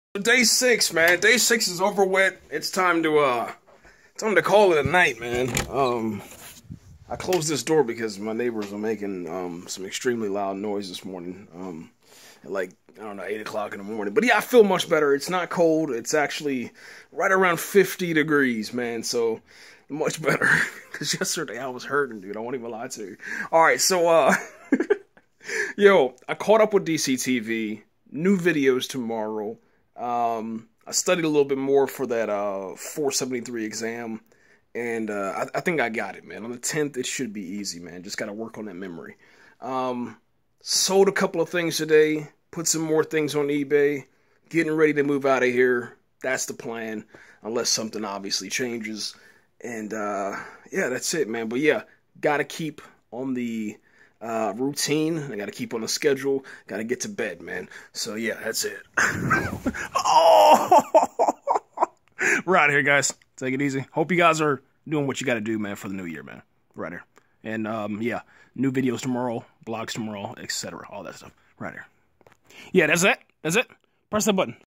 VLOG - Day 6: Healthy Day, 473 Memory, Video Update & Routine
DAY 6! FEELING MUCH BETTER! WARM WEATHER!!!

ALREADY THE 5TH OF JANUARY! 2018!

I think I'll continue this trend.

Translucent Origin will sponsor your channel to help you get more views and subscribers.

FAN MAIL: COMING SOON
(Updated: January 1, 2018)